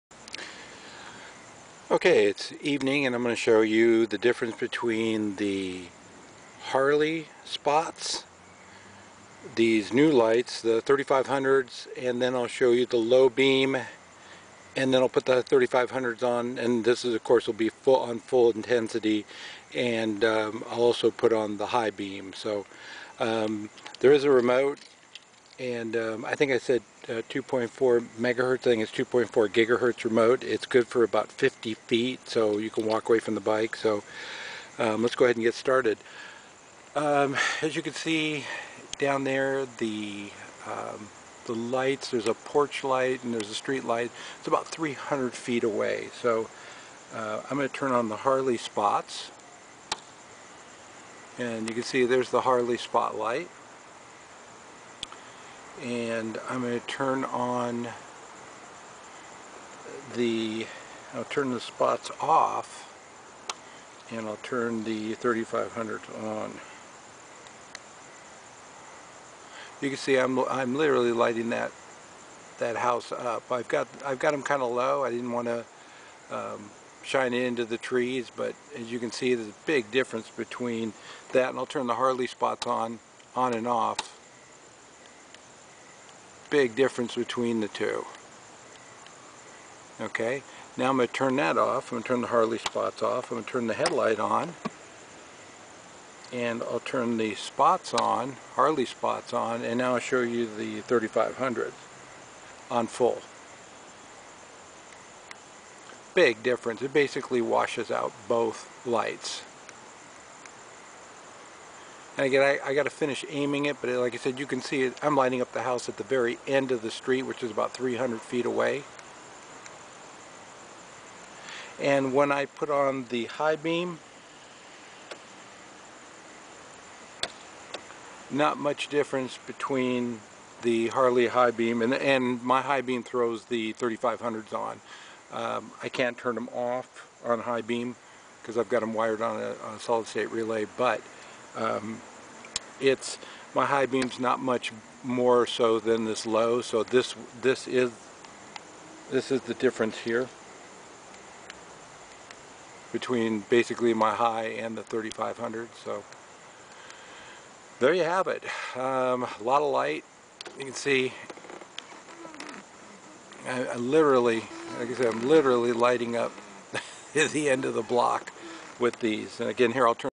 3500LM LED Lighting on a 2012 Harley Ultra
This is to demonstrate the intensity of these small LED lights I just instated on my bike. One light is brighter than all 3 of my Harley LED lights and that's what I'll be showing. Been running them for over a year now and very happy with them